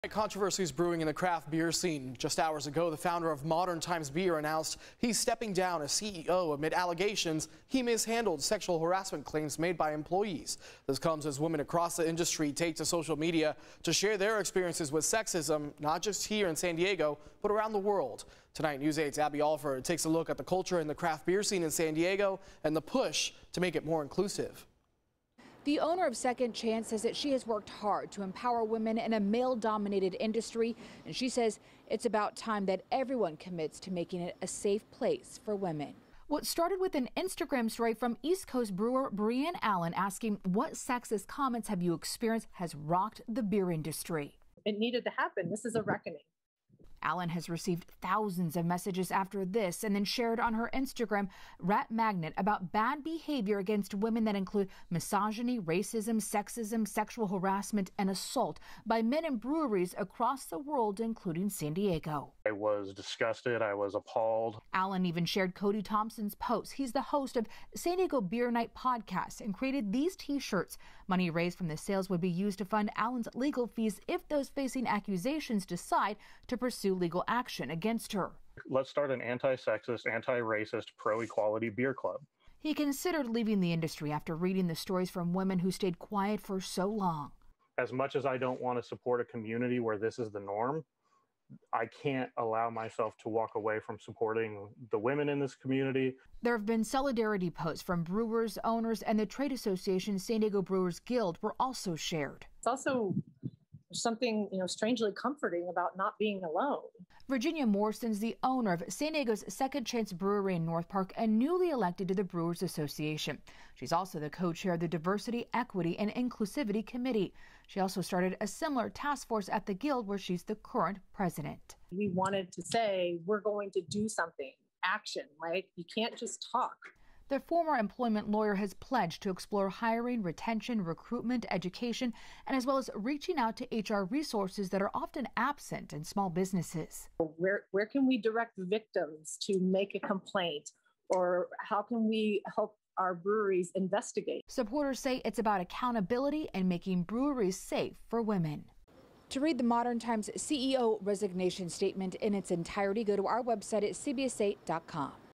San Diego's Modern Times CEO steps down amid sexual misconduct allegations
In the wake of allegations of sexism, misogyny, harassment, and assault perpetrated against women in the brewing industry a San Diego CEO is stepping down.

Tuesday night, Modern Times Founder and CEO posted an online statement that he would be resigning after allegations of misconduct.